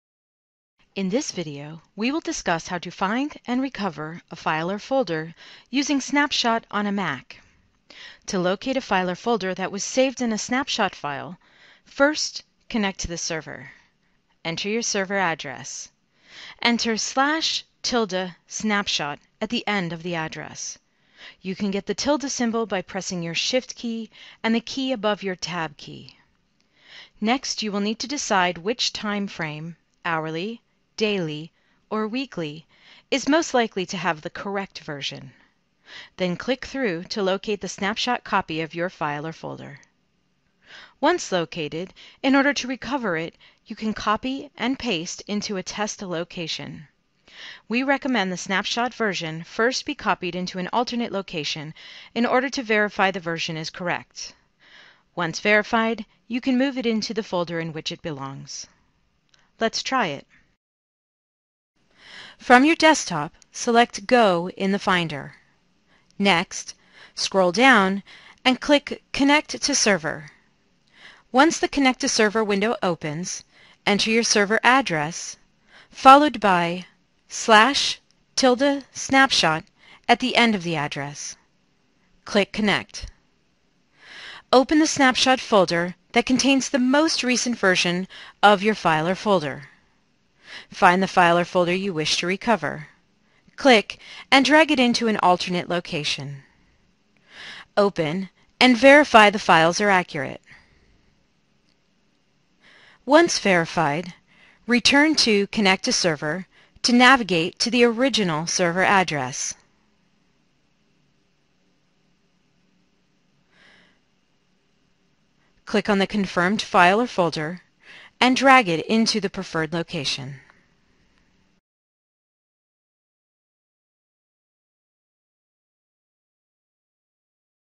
Snapshot on a Mac: Find and Recover a File or Folder
Snapshot is a backup program for Q/R/P drive. If you accidentally delete a file or folder in one of these network drives, you can use Snapshot to recover it. This video walks through recovering a network file with Snapshot on a mac.